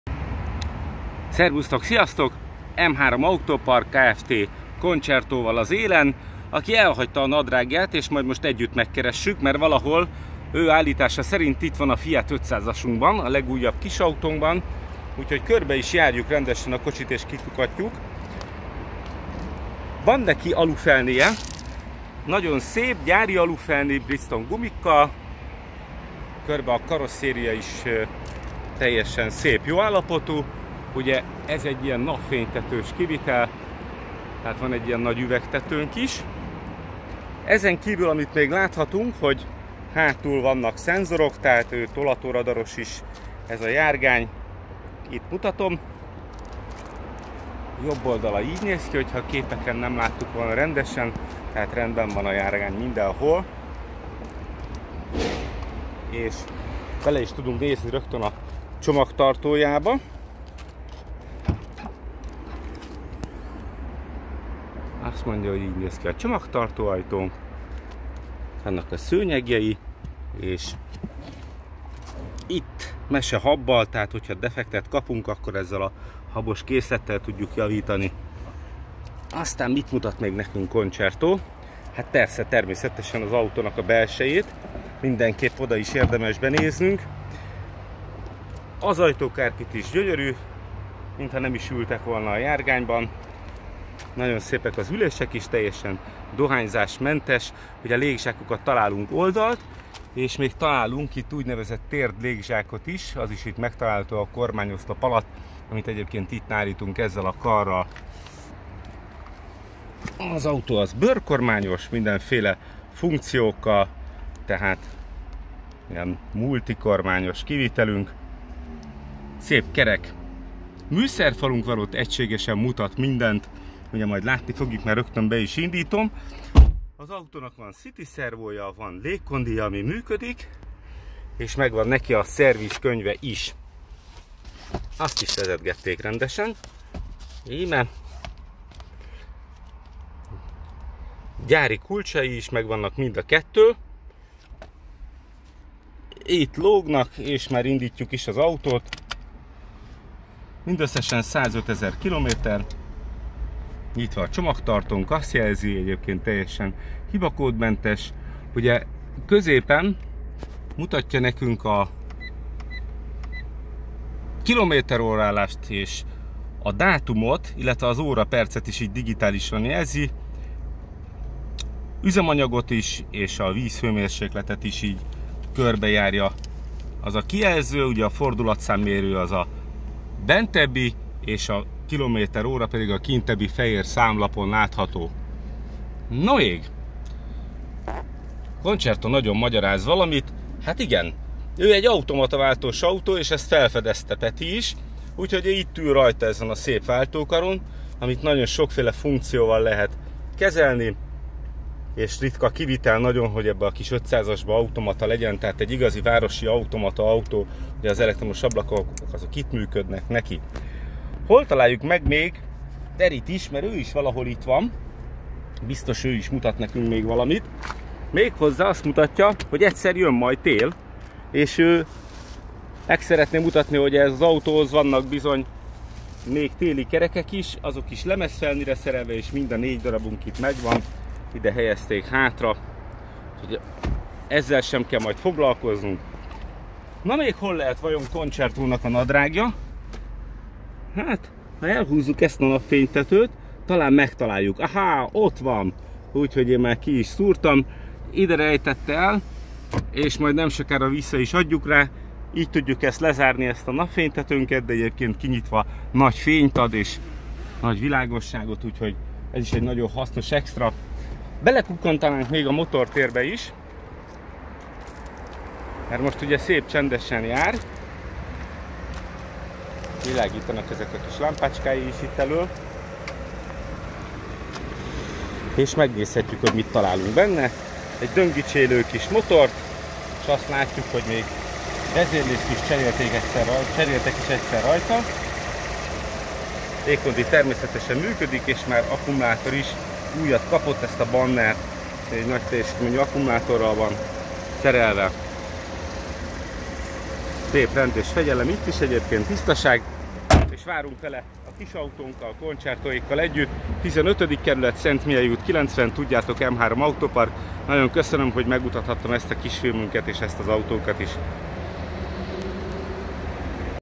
Fiat 500 1.2 8V Lounge Dualogic Automata
Keveset futott vezetett szervizkönyves Fiat 500 eladó 1.2 benzines 69 lóerős motorral, tökéletese Dualogic automata váltóval, legmagasabb Lounge felszereltséggel, gyönyörű fehér karosszériával, kopás és dohányzásmentes utastérrel. Jéghideg klímával! 2 db eredeti gyári kulccsal, 2 év friss műszaki vizsga!